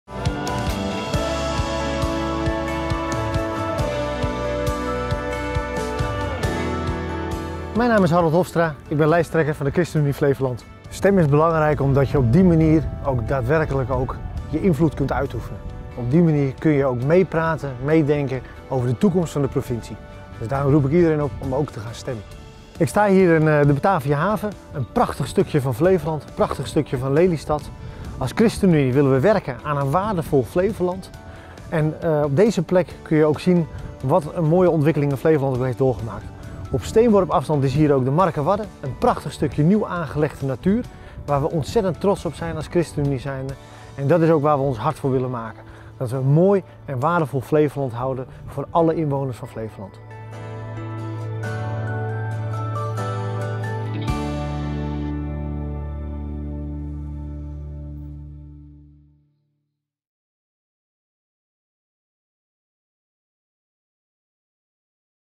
Provincie Flevoland - ChristenUnie
Verkiezingsvideo van de ChristenUnie Flevoland voor de Provinciale Statenverkiezingen van 2019. Meer weten over deze verkiezingen? Ga naar